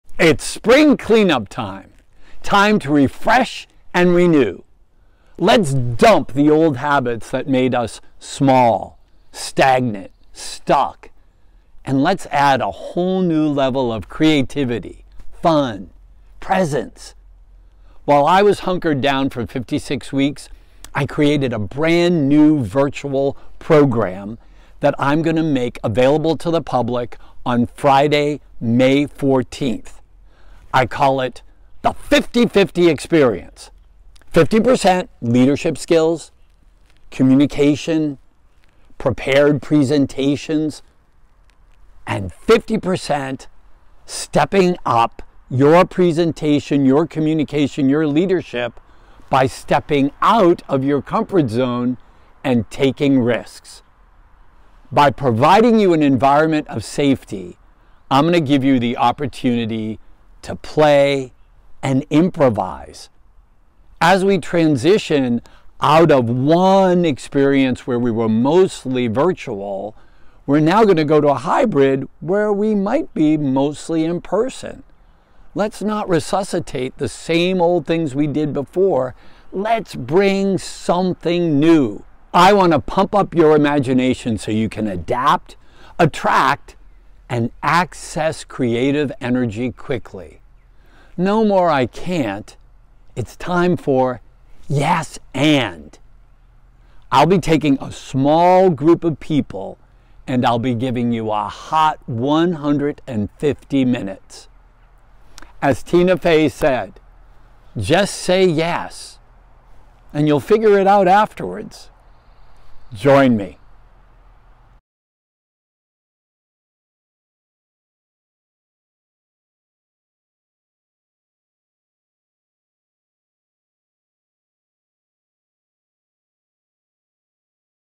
Michael Allosso | Improve Yourself with 50-50 Experience Rule
Michael Allosso, a self-awareness specialist and motivational speaker, is all ready to launch his brand-new virtual program "50-50 Experience". It's spring clean-up time, so refresh yourself and renew the old habits that made you small, stagnant, and stuck. Add a new level of creativity and fun to your current life. Let’s not resuscitate the same old stuff we did; bring something new in life. Providing you an environment of safety and giving you the opportunity to improvise. Here in this virtual program, you will learn 50% leadership, communication, prepared presentation skills. And, 50% will focus on stepping out of your comfort zone and taking risks. This will be a small intimate gathering

Pumping your imagination so you can adapt, access, and attract creative energy quickly. It's time to say, "YES I CAN." Join him on the 14th of May, Friday!

𝐖𝐚𝐭𝐜𝐡 𝐭𝐡𝐞 𝐨𝐭𝐡𝐞𝐫 𝐡𝐞𝐥𝐩𝐟𝐮𝐥 𝐯𝐢𝐝𝐞𝐨𝐬 𝐨𝐧 𝐭𝐡𝐞 𝐜𝐡𝐚𝐧𝐧𝐞𝐥 𝐚𝐧𝐝 𝐚 𝐟𝐞𝐰 𝐨𝐟 𝐭𝐡𝐞 𝐯𝐢𝐝𝐞𝐨𝐬 𝐚𝐫𝐞 𝐥𝐢𝐧𝐤𝐞𝐝 𝐛𝐞𝐥𝐨𝐰: